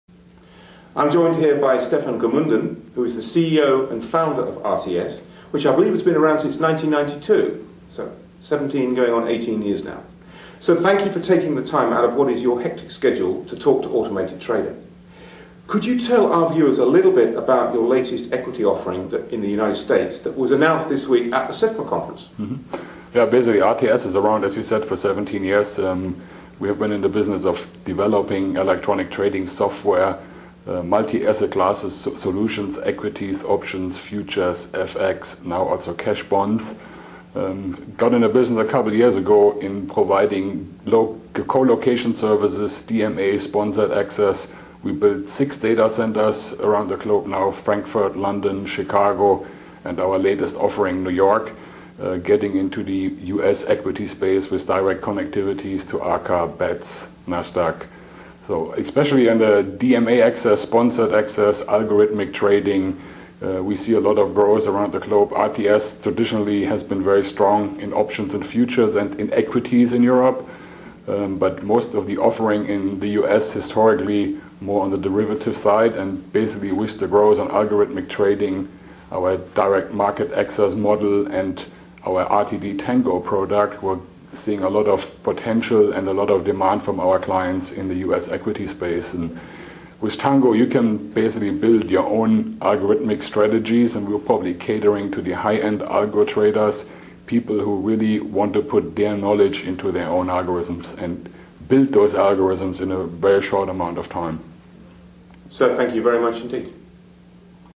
RTS CEO and Founder Steffen Gemuenden talks to Automated Trader
With cutting elge algorithmic trading tools now a key alpha generation dependency for many buyside firms, RTS CEO and Founder Steffen Gemunden talks to Automated Trader about RTS's algorithmic trading tool RTD Tango, and their latest developments to extend Direct Market Access (DMA) and Sponsored Market Access (SMA) capabilities to US equity markets.